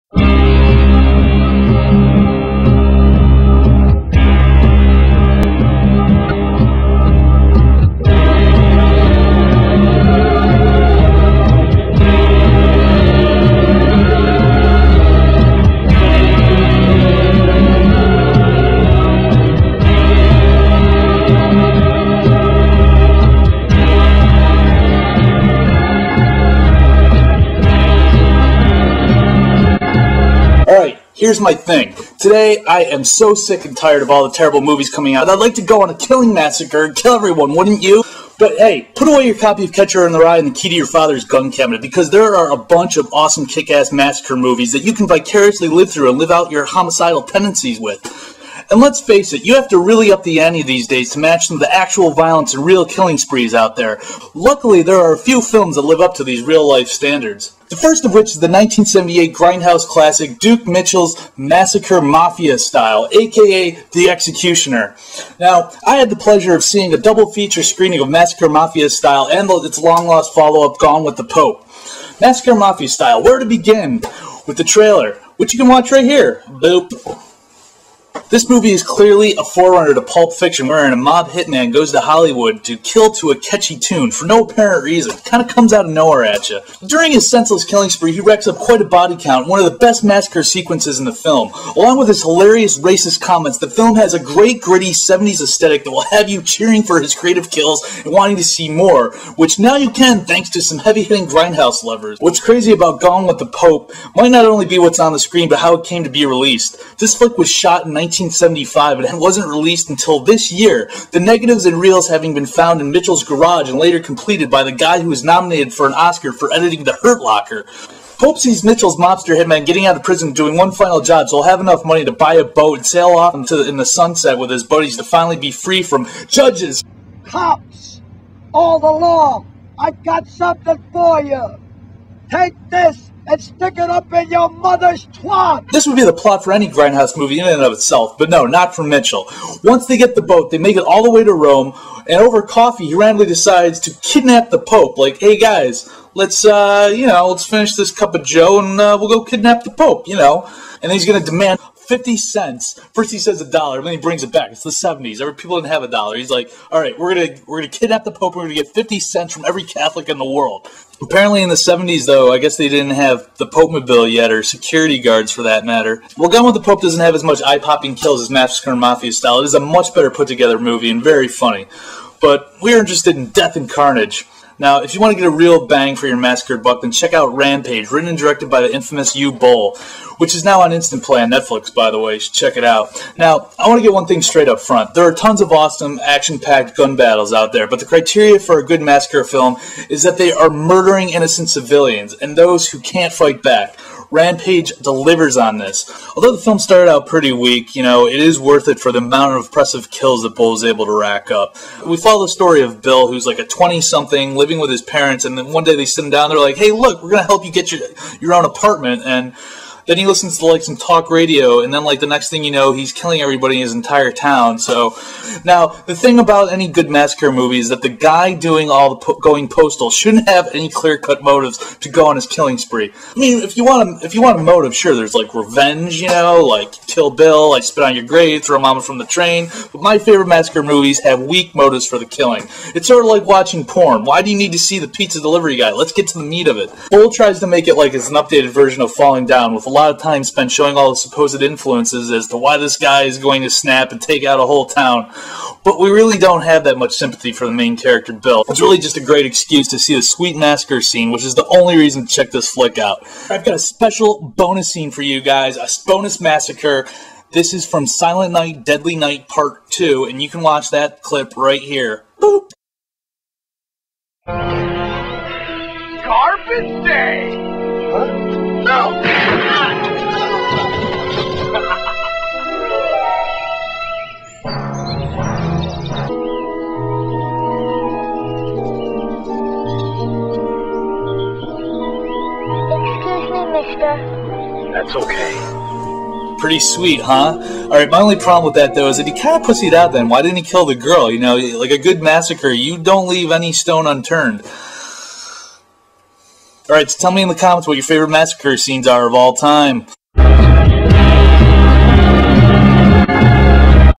Massacre Movies! - The Daily Grindhouse EP #1
In this episode of The Daily Grindhouse Phil reviews grindhouse classic Massacre Mafia Style along with its long lost follow up Gone With The Pope!

"We Watch These Movies So You Don't Have To!"